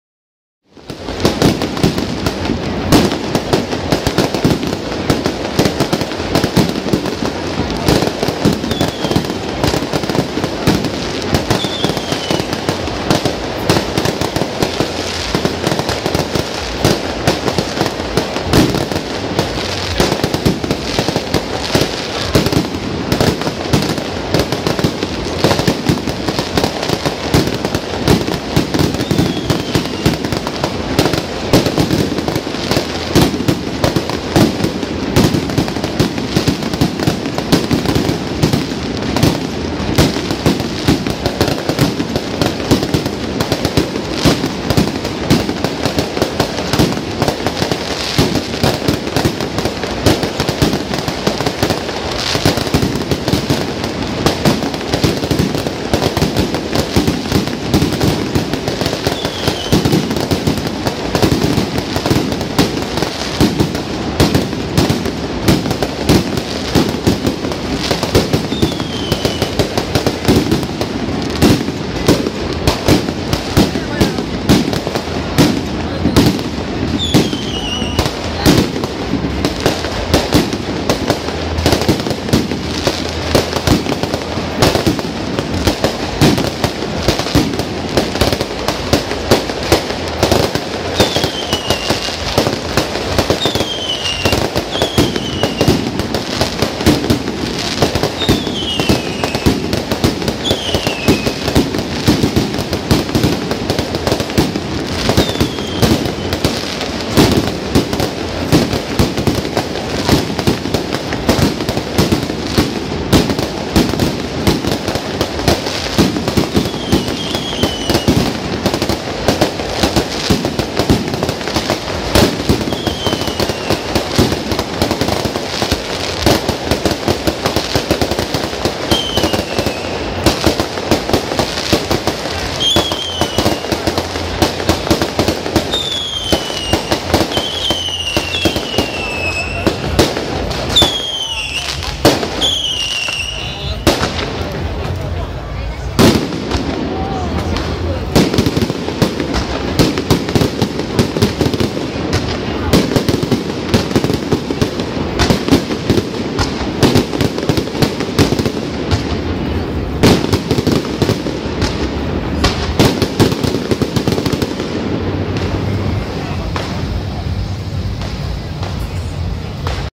104乙未年艋舺青山宮正日遶境~《帝爺會辣炮恭迎青山宮靈安尊王》 1
2015/12/3艋舺青山宮靈安尊王聖誕正日遶境